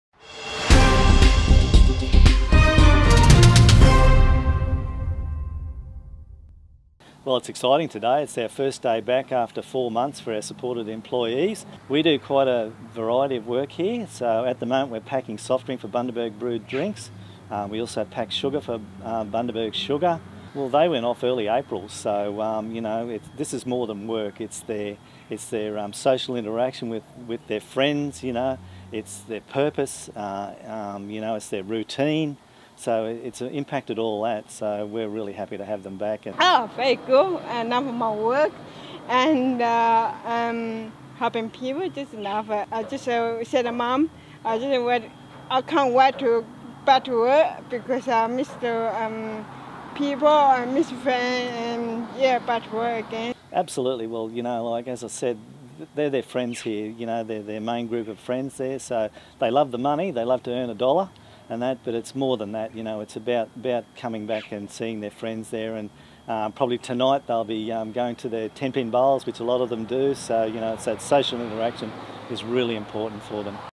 Endeavour Foundation Team Back at Work
More than 60 Bundaberg Endeavour Foundation employees returned to work today (20 July) for the first time since COVID-19 restrictions were imposed four months ago.